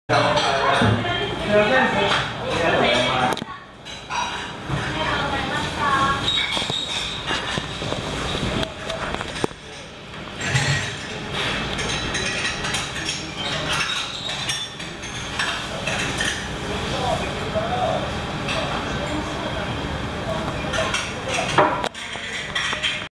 とんこつラーメンを福龍閣島田店で
福龍閣の島田店でとんこつラーメンを頂きました、
このお店のとんこつラーメンはとても美味しいですね。
豚骨の匂いも味も抜群でした。